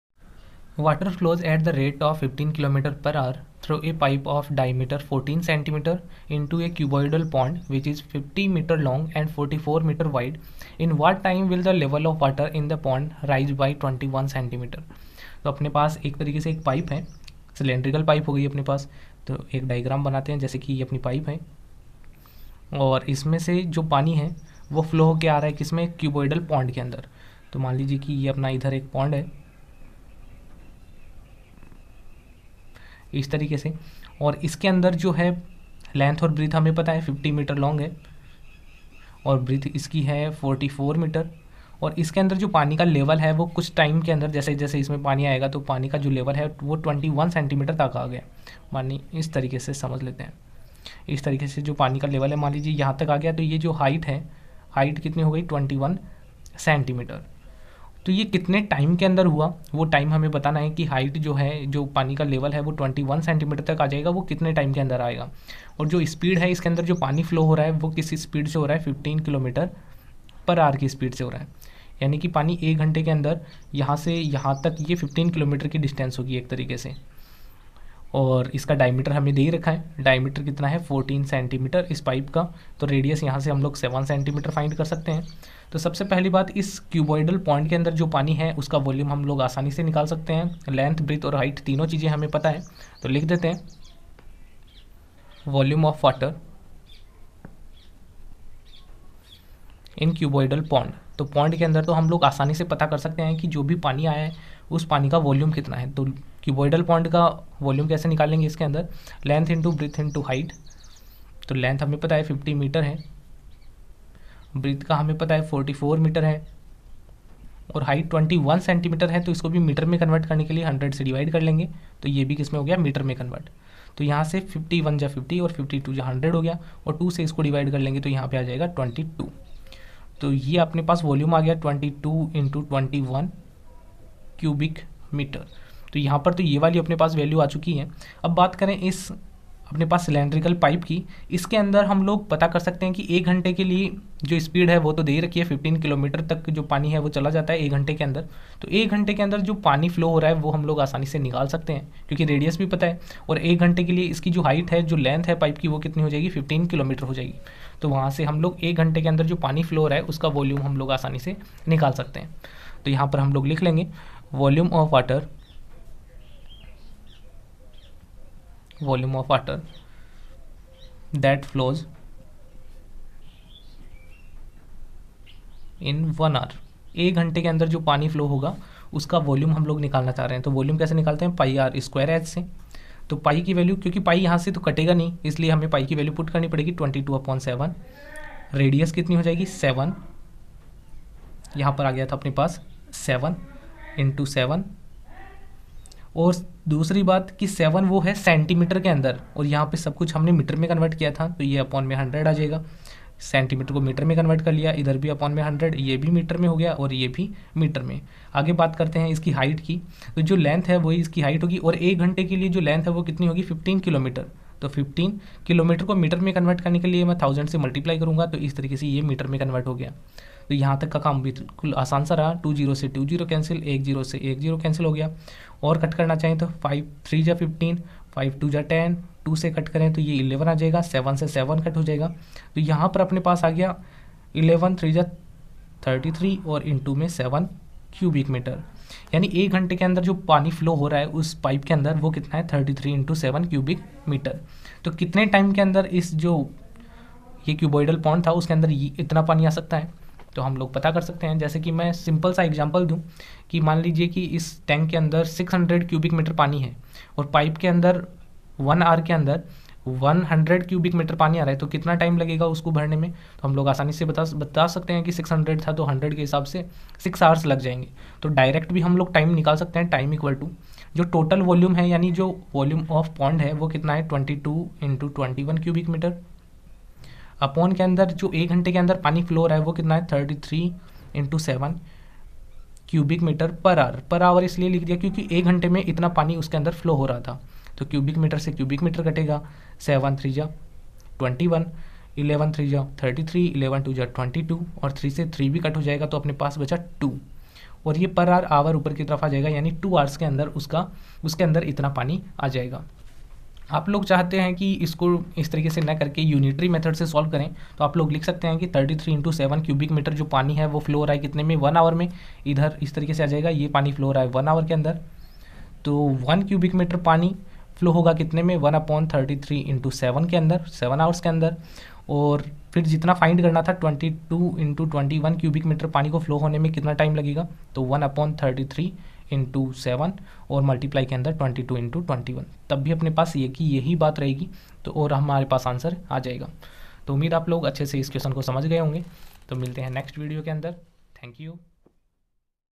Water flows at the rate of 15 km/hr through a pipe of diameter 14 cm into a cuboidal pond which...
Water flows at the rate of 15 km/hr through a pipe of diameter 14 cm into a cuboidal pond which is 50 m long and 44 m wide. In what time will the level of water in the pond rise by 21 cm?